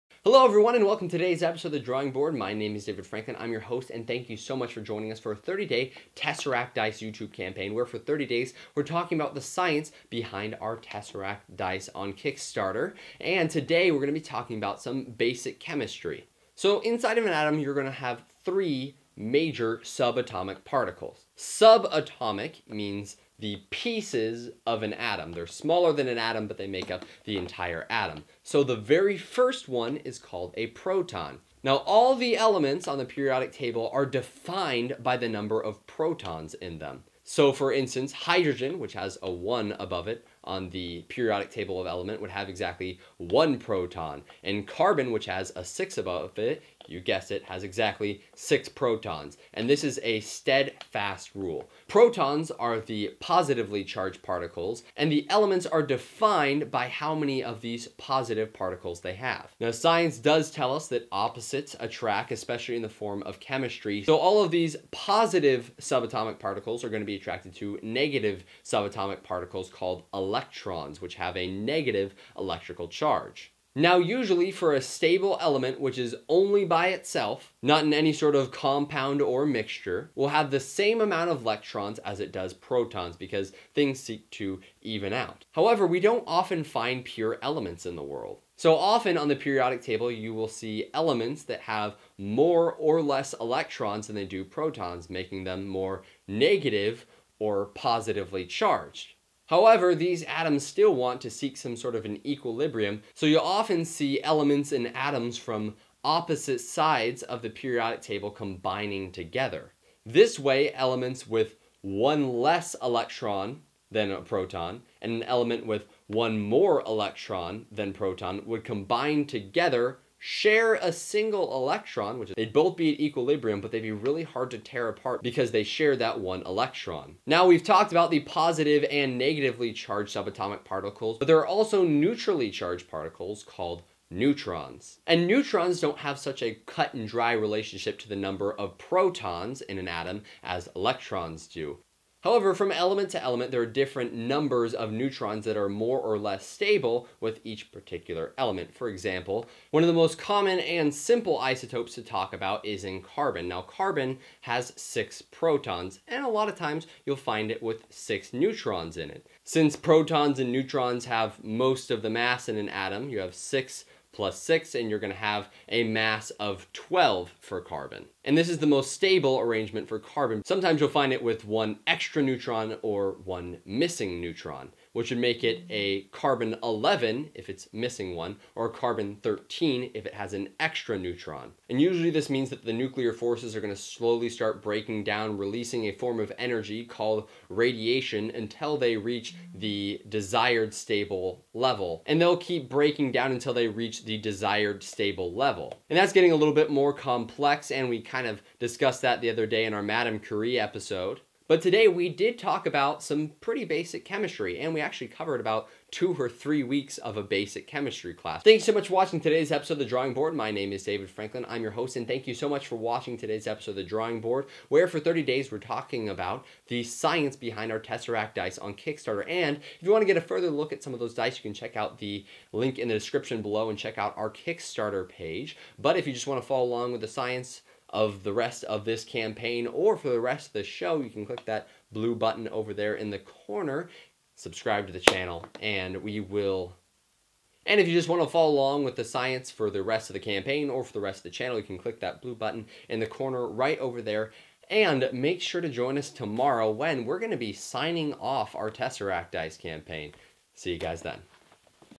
Sub-Atomic Particles: Protons, Neutrons, Electrons: Basic Chemistry!
Today David talks about Sub-Atomic Particles: Protons, Neutrons, Electrons: Basic Chemistry!